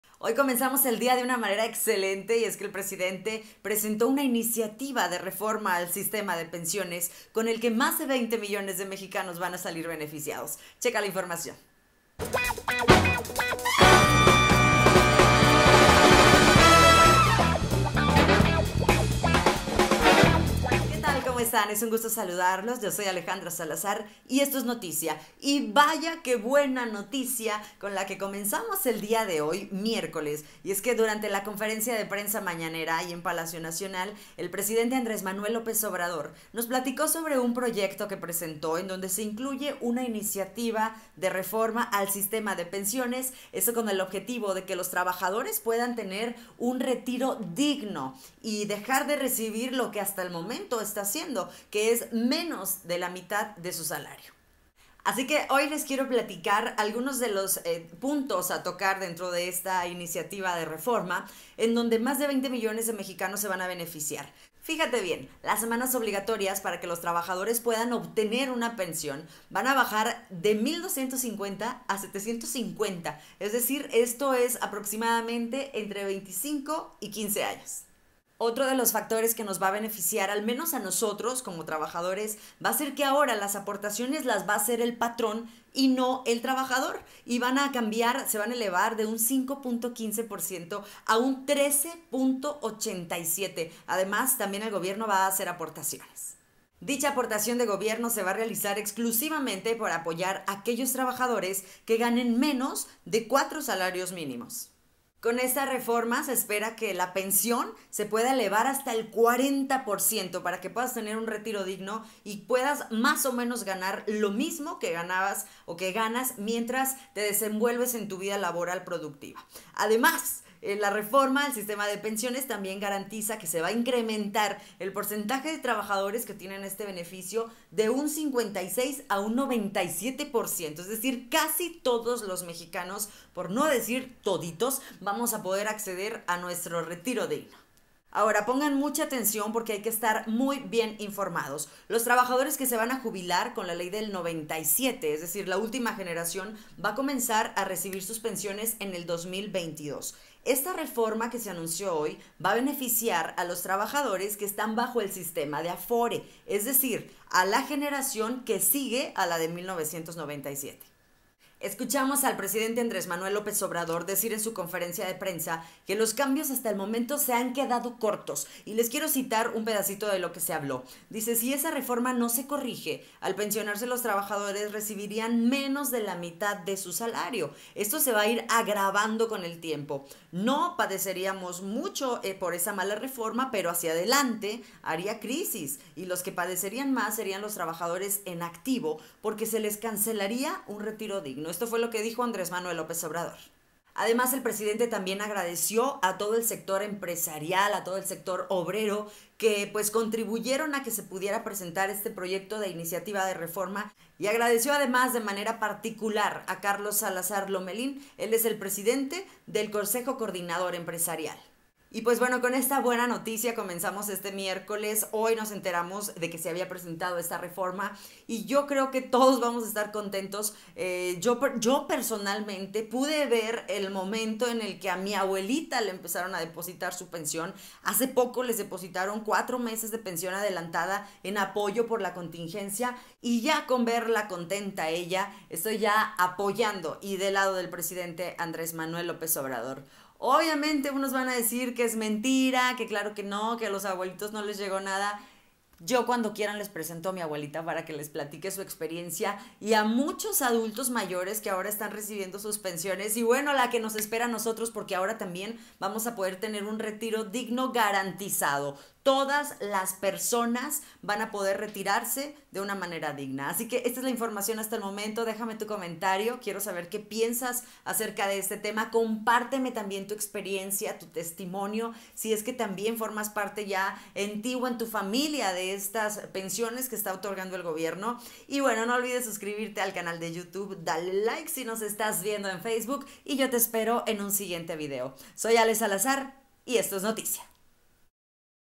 Aumentarán 40% de las pensiones a trabajadores para un retiro digno
Entérate!! Aumentarán 40% de las pensiones a trabajadores para un retiro digno